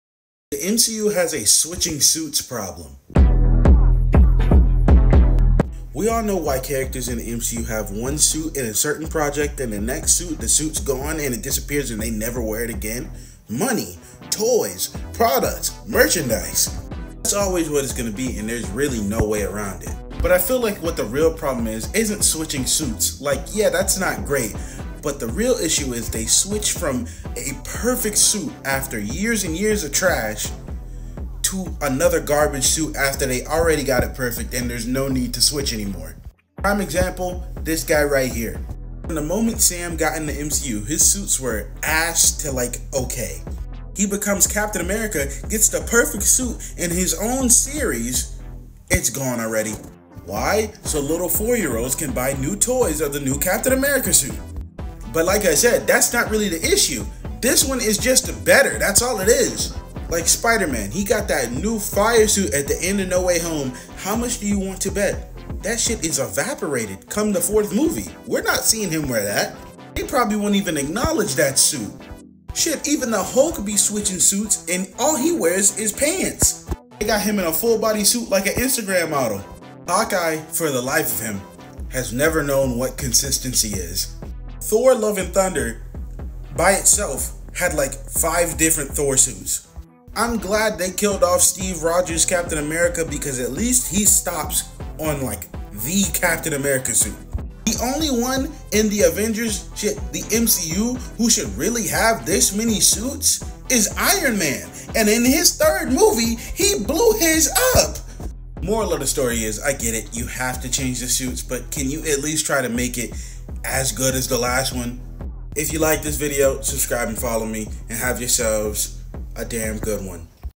THE MCU NEEDS TO STOP SWITCHING SUITS!
The mcu be switching the best suits for no good reason